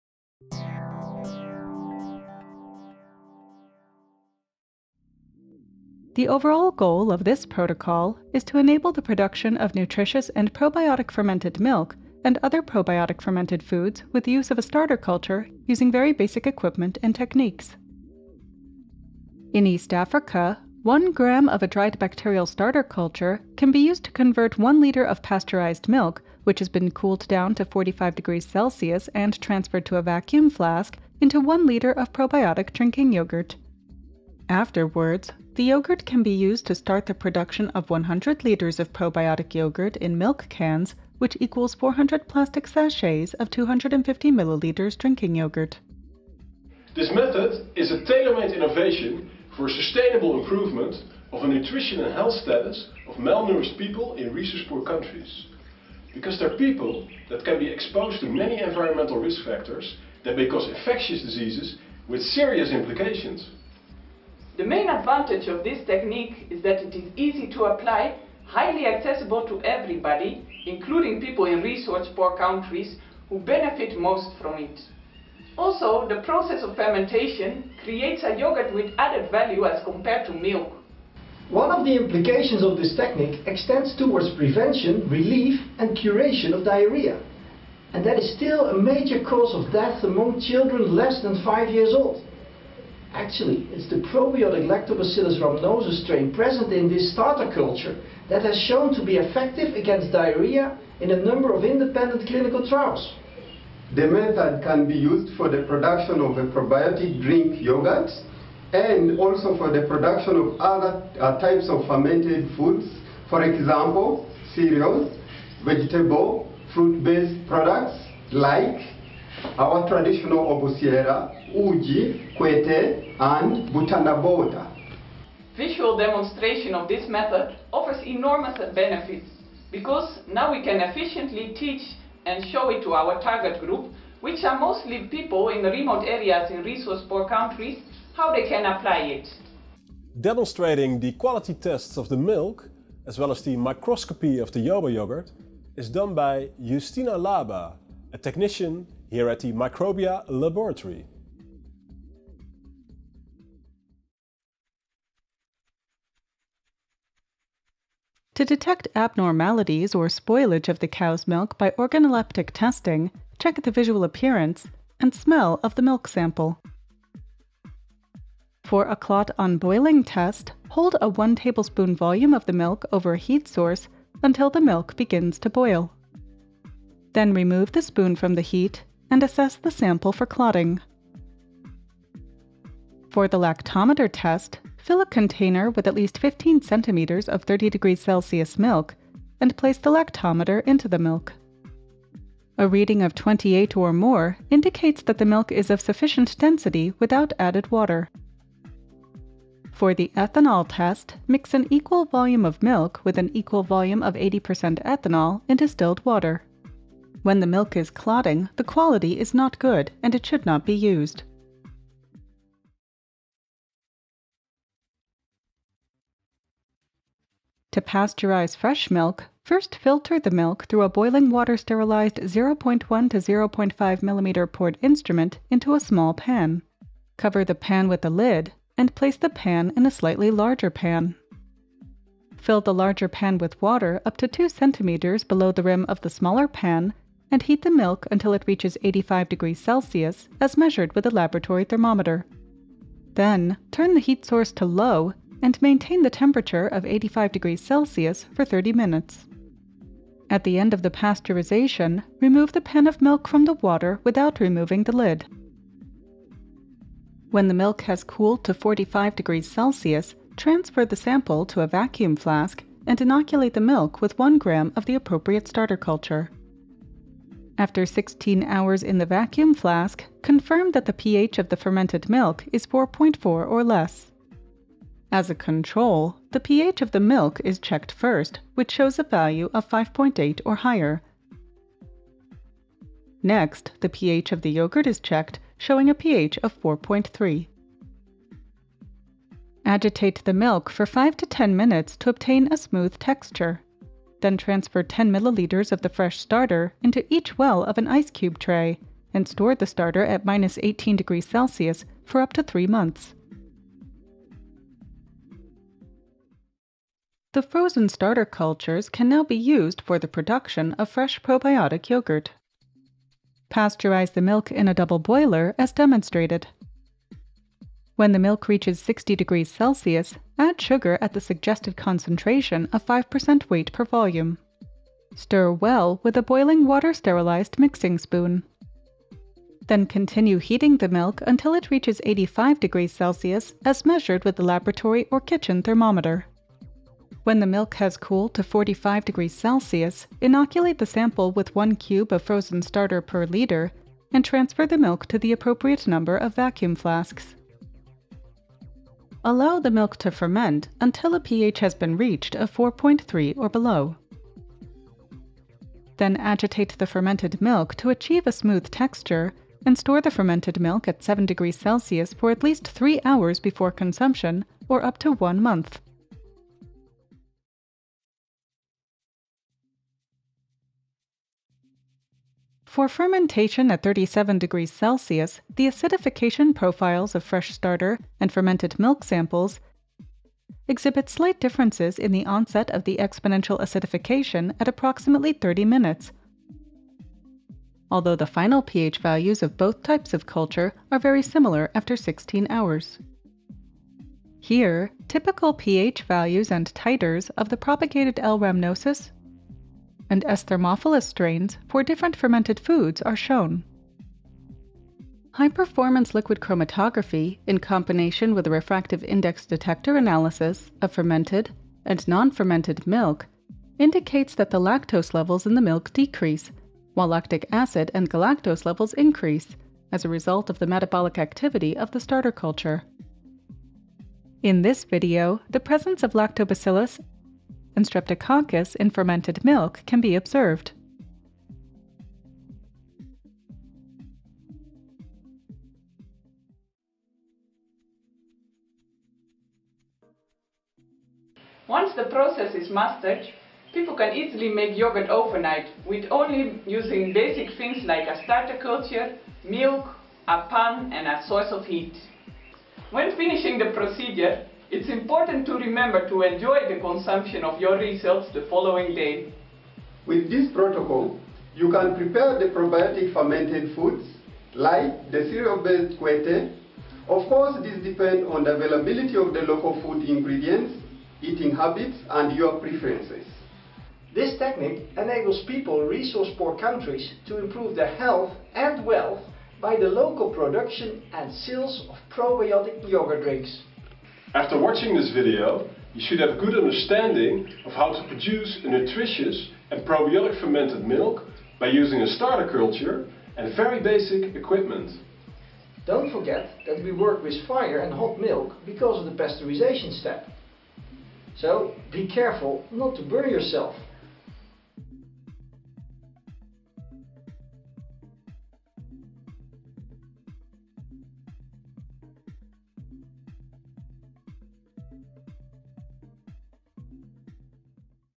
Novel Production Protocol for Small-scale Manufacture of Probiotic Fermented Foods
An Open Access Protocol from JoVE Biology.

A production protocol is described for the small-scale production of probiotic fermented dairy drink with the aid of a novel bacterial starter culture.